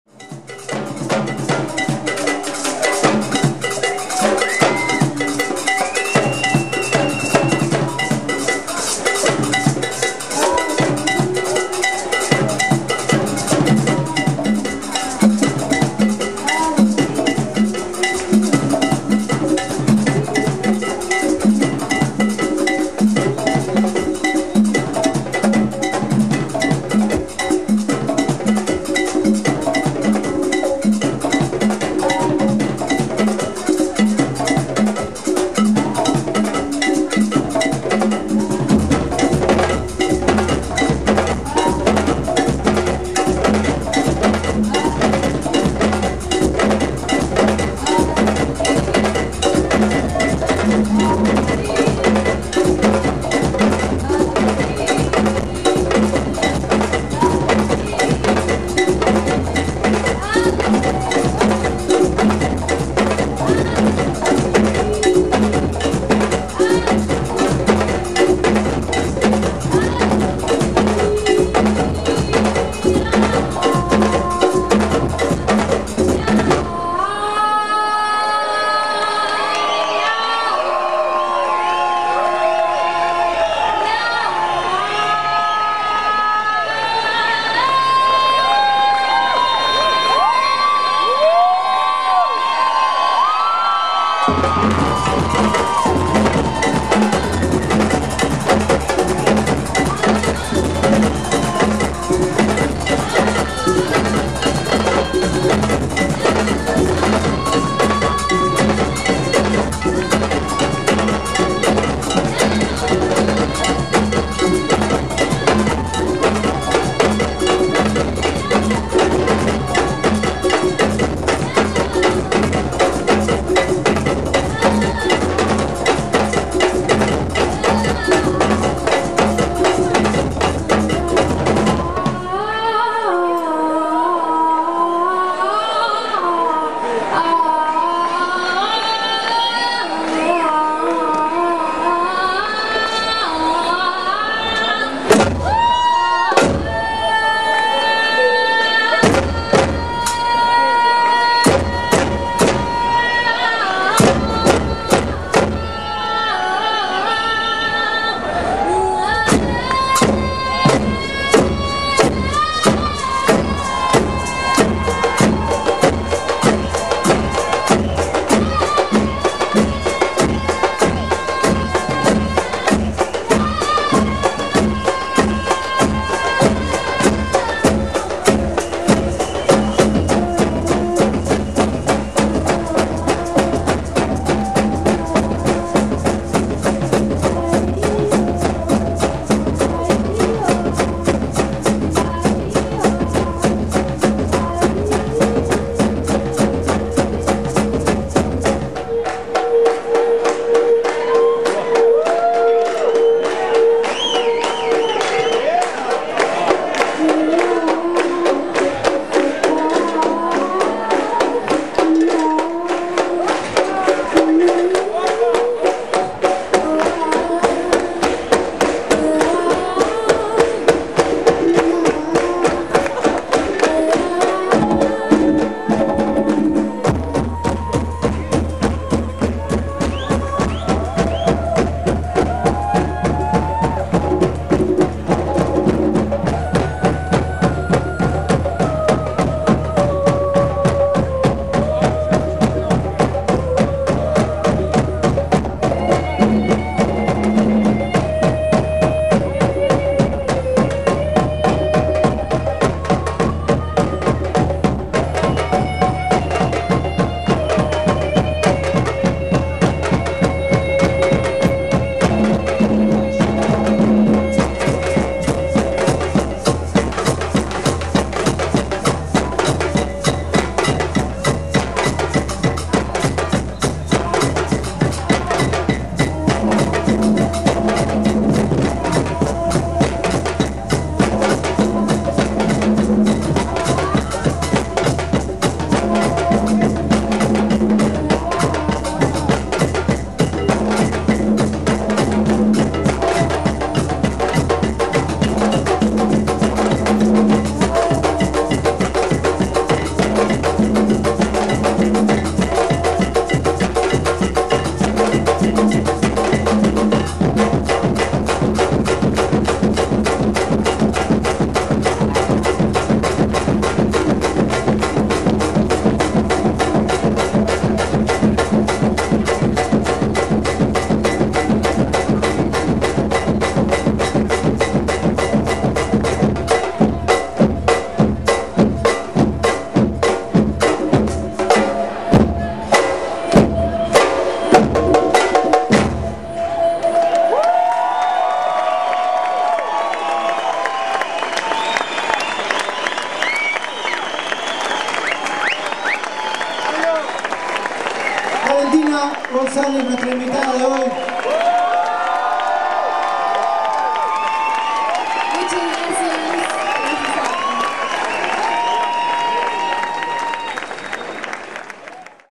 VALENTINA Improvisando en Vivo con la Bomba de Tiempo
Valentina con la Bomba de Tiempo en el Konex, Buenos Aires, Argentina,10 de enero 2011
(Editado por Mr.Miguelius y grabado por Eliana Arévalo)